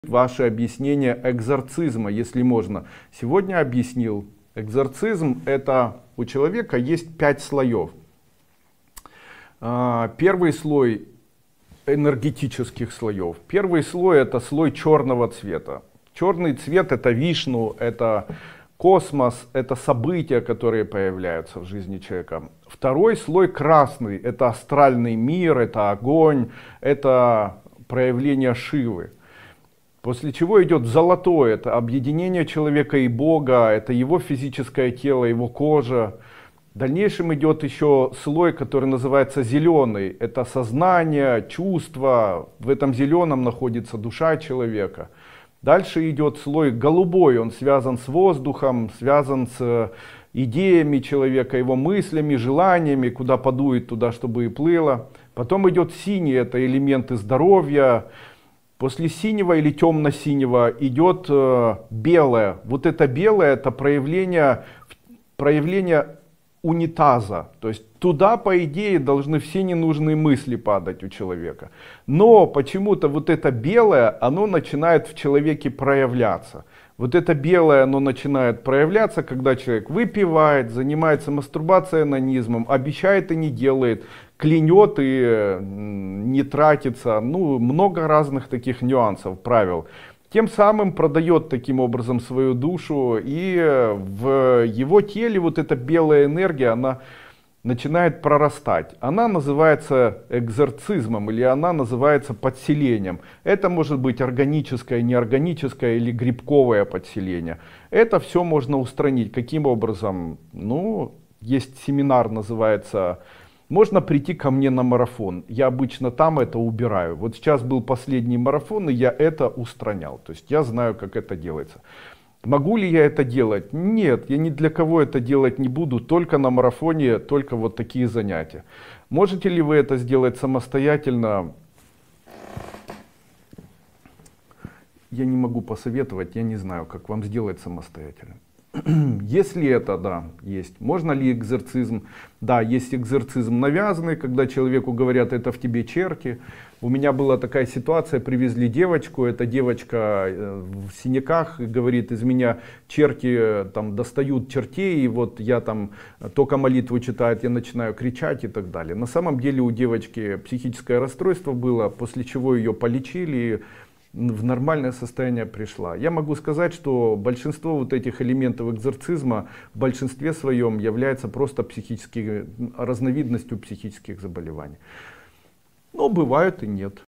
Что такое экзорцизм? @Андрей Дуйко
Что такое экзорцизм?
и потом идет белый слой, туда должны падать все ненужные мысли человека; но обычно это белое, начинает в человеке проявляться, когда человек выпивает, занимается онанизмом, обещает и не делает, клянет и т.п., тем самым человек продает свою душу и это белая энергия начинает прорастать и эта энергия называется экзорцизмом или подселением ; есть экзорцизм навязанный и в основном это разновидность психических заболеваний, но не всегда.